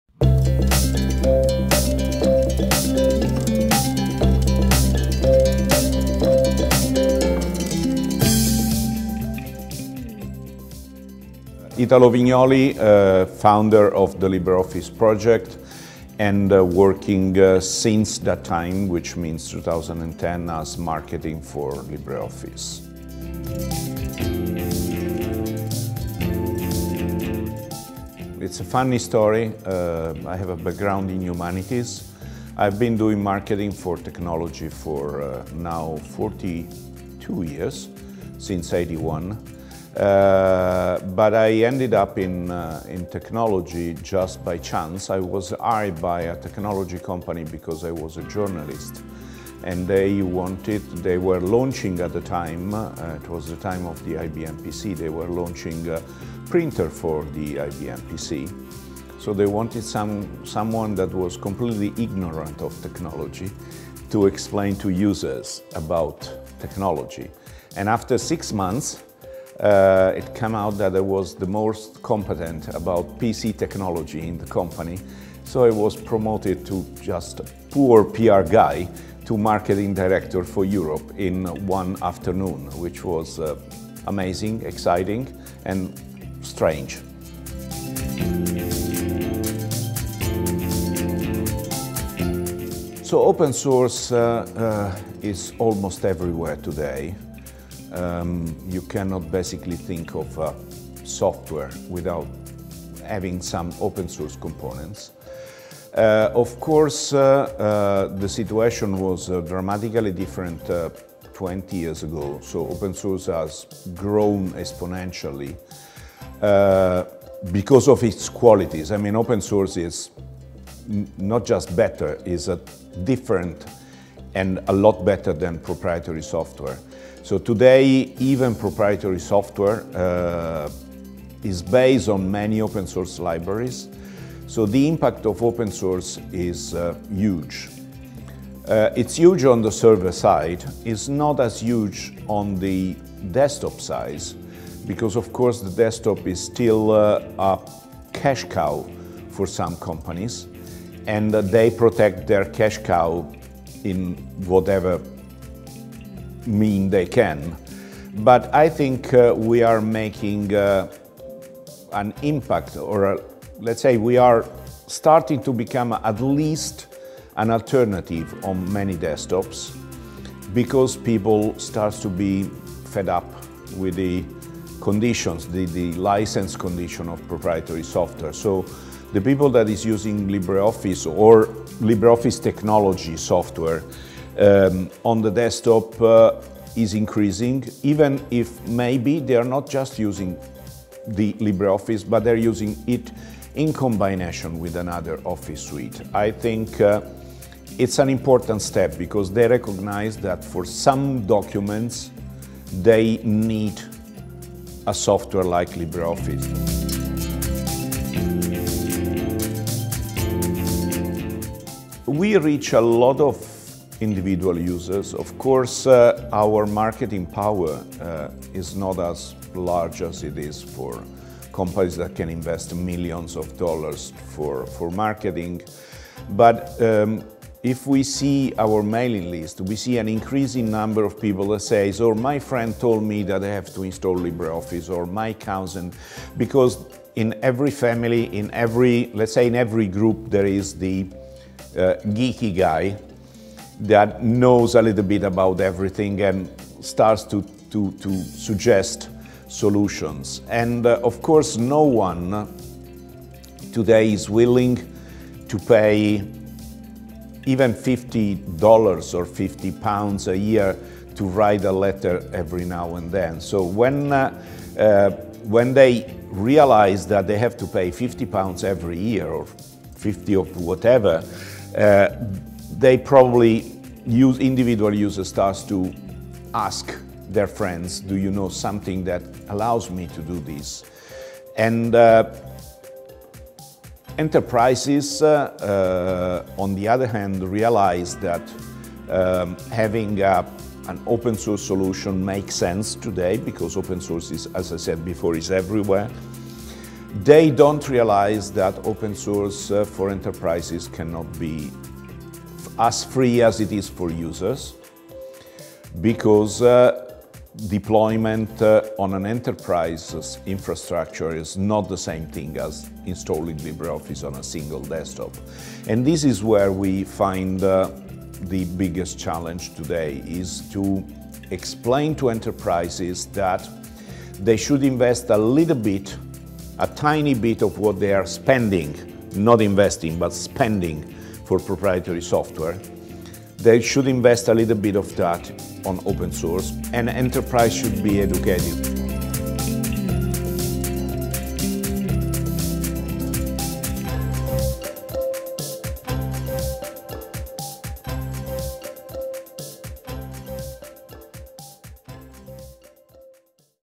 Collabora Online & LibreOffice - Interview with Italo Vignoli (Part 1 of 2)
A great insight into how the open-source world has developed from our good friend Italo Vignoli. Find out why open source is expanding so rapidly, and how we can further our reach whilst proprietary companies spend millions to maintain their monopolies.

00:00 Intro
00:25 How did you first get involved with LibreOffice?
01:30 What impact do you think that open-source software is having across Europe?
03:30 How can we further the reach of open-source software?